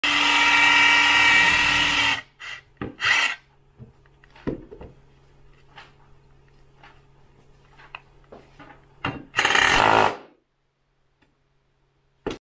BHC18 también sin percusión para perforar por ejemplo azulejos
Martillo perforador BHC18|  Potencia compacta. Taladra a máximo rendimiento
También sin percusión: también se puede desconectar la percusión, p. ej., para perforar azulejos